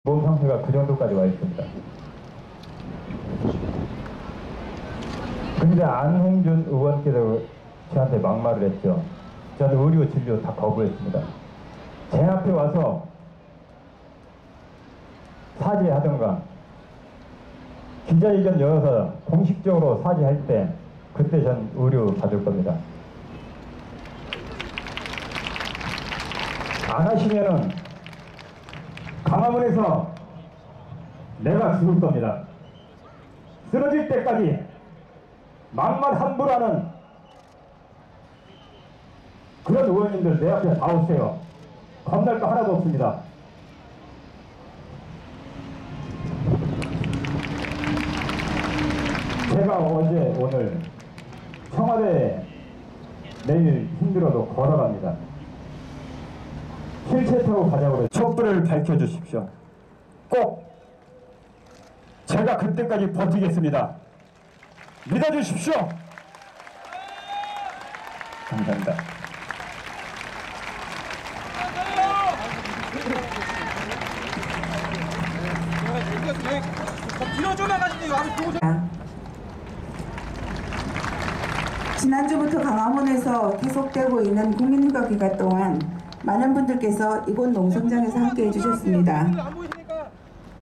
김영오 유민아빠님
세월호 참사 116일째 단식 27일째 중인 유민 아빠 김영오님의 연설